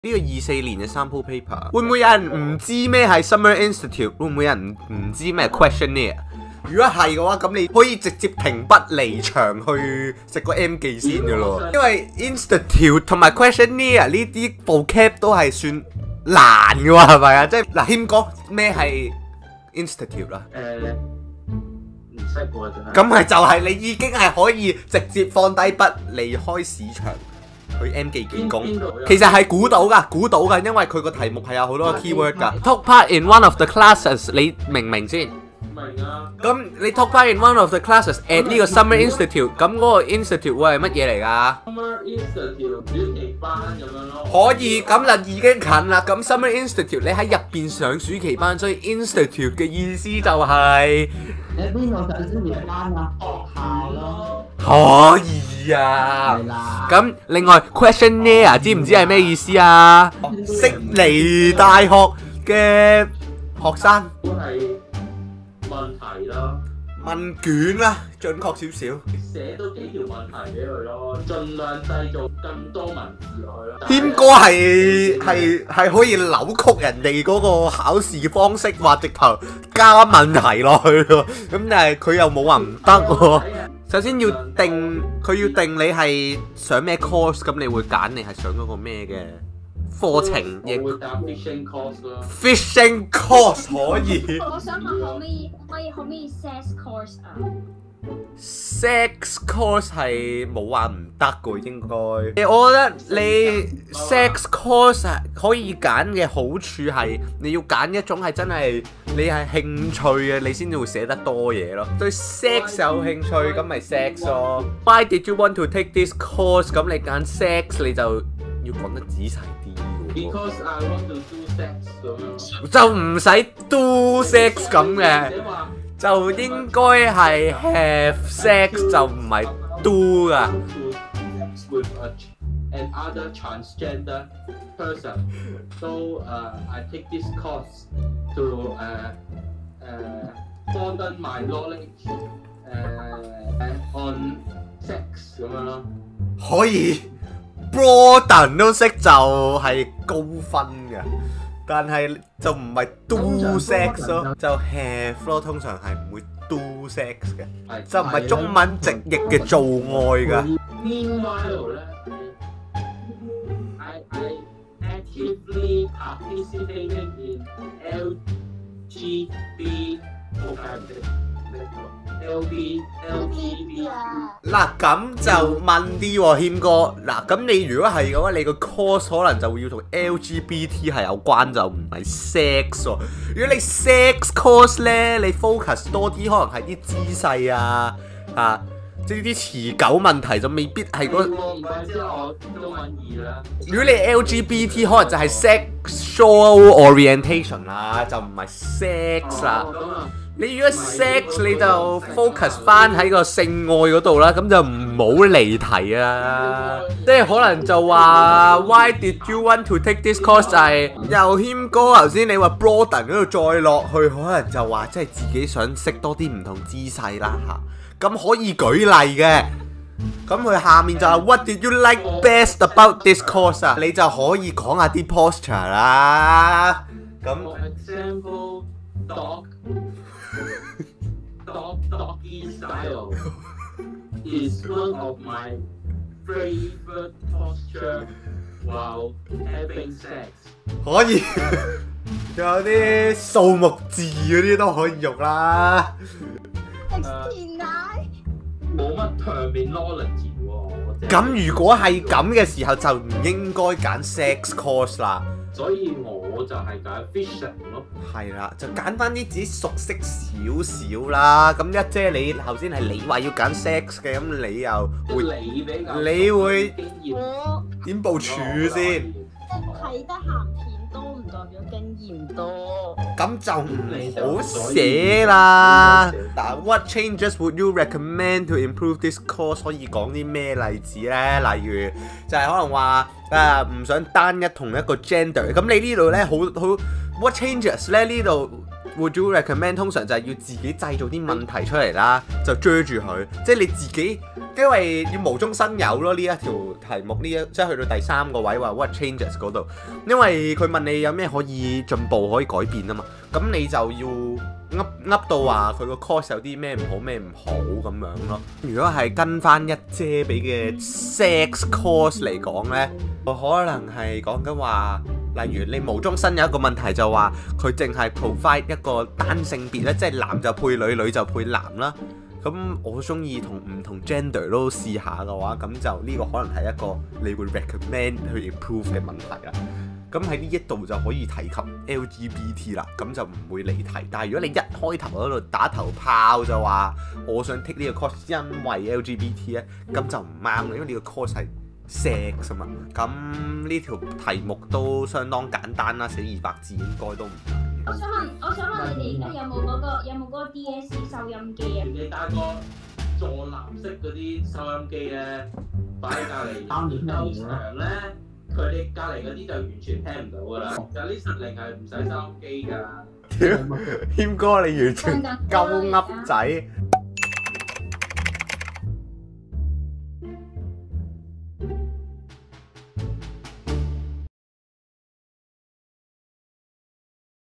【廢J Live精華】悉尼大學生🇦🇺試做DSE英文作文📖 - 廢J補習台 ｜謙哥 J 牙童 Ah Ma
星期一、五出正片🎬 Live精華不定期放送🔴